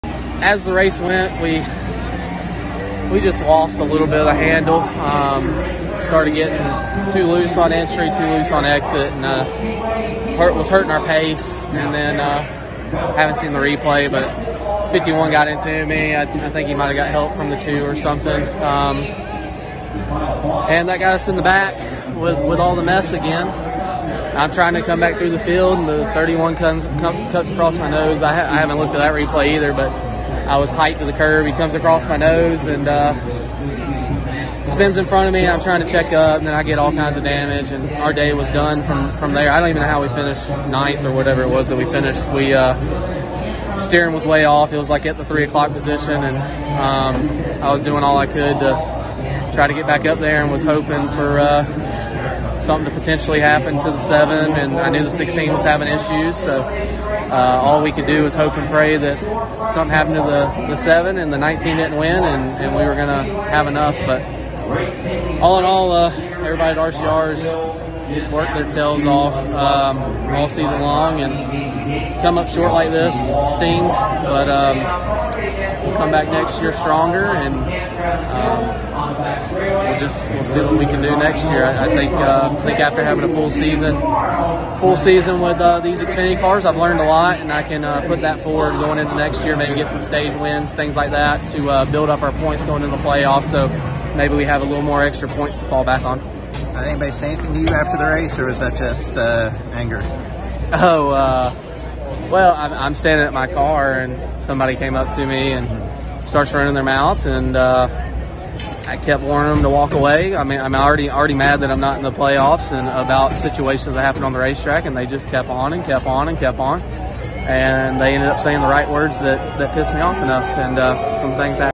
“He said the things that pissed me off and things happen” Austin Hill on fight with Myatt Snider
NASCAR Austin hill on fight with Myatt snider Martinsville Xfinity race.